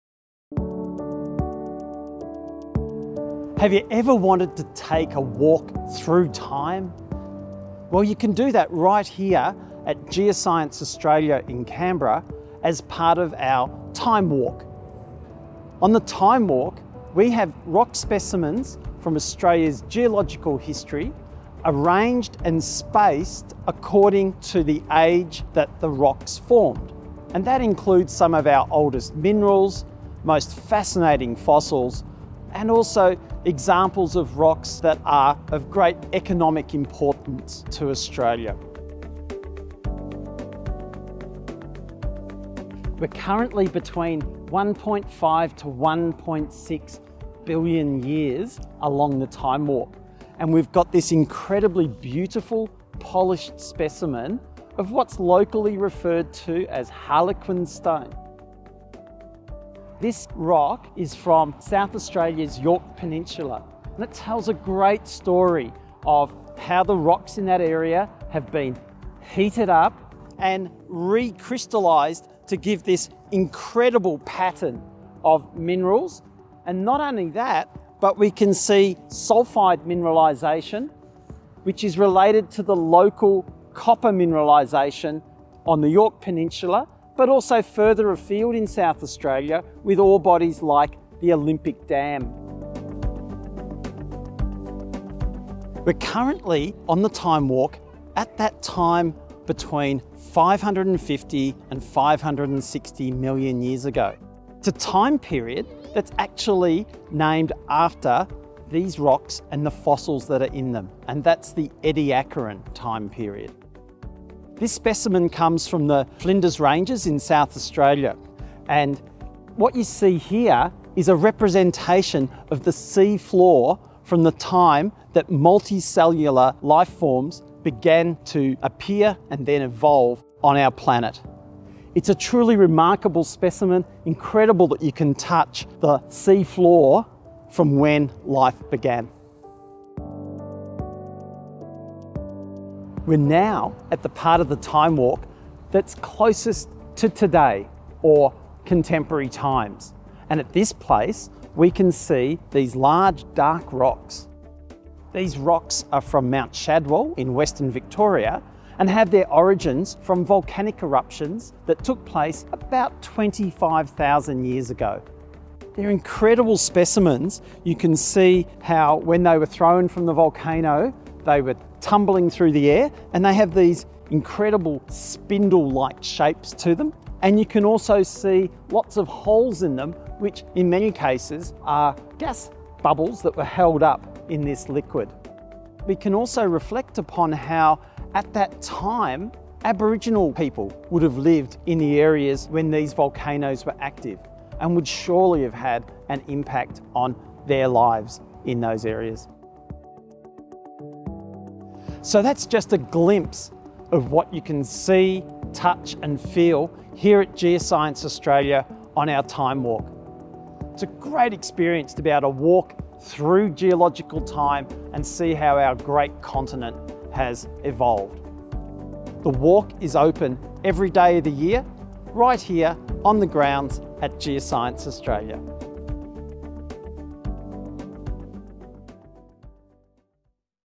Geoscience Australia's Geological Timewalk
Geoscience Australia's Geological TimeWalk is a physical representation of geological time in the landscaped gardens of Geoscience Australia. The TimeWalk leads you on a 1.1 kilometre journey through the Earth's 4600 million year (Ma) history and represents the evolution of the Earth from its formation though to the present day. Since the TimeWalk's installation in Canberra in 2009, large rock samples have been collected from around Australia and put on display at locations along the TimeWalk that reflect their age. Each rock has a story to tell, with geological, economic, environmental and/or cultural significance.